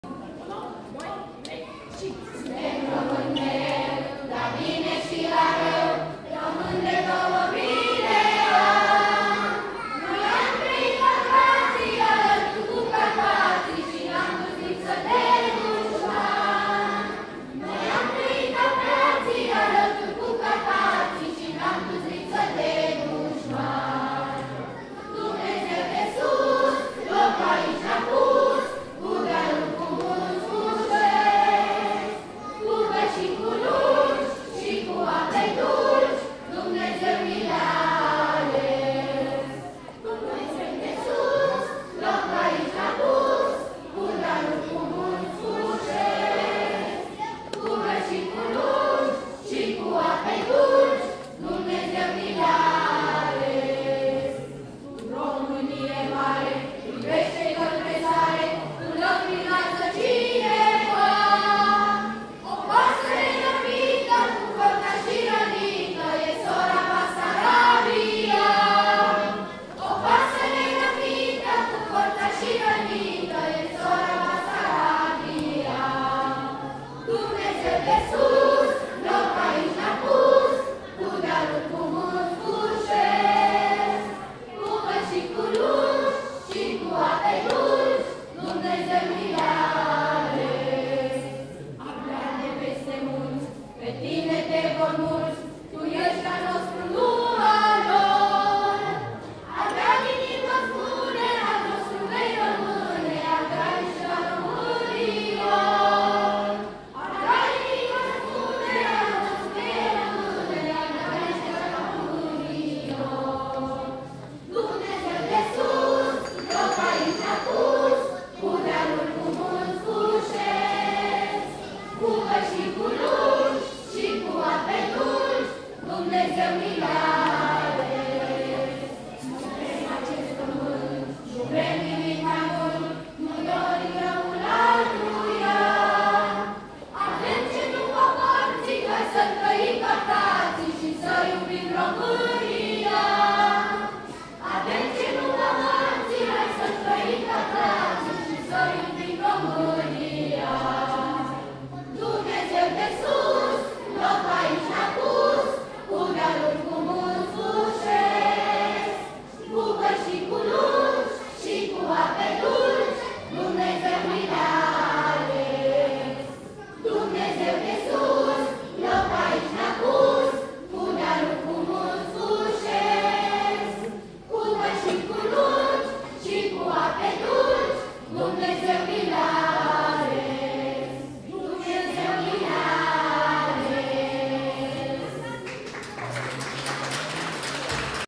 (43) Cântare - Școala Brâncovenească: Serbare de sfârșit de semestru (2023.11.29, Restaurant Dorna)
Pe 29 noiembrie 2023, de la ora 16:00, a avut loc serbarea de sfârșit de semestru - Școala Brâncovenească (Școala "Sfinții Martiri Brâncoveni").

Evenimentul a avut loc la Restaurant Dorna, Stațiunea Mamaia, Constanța.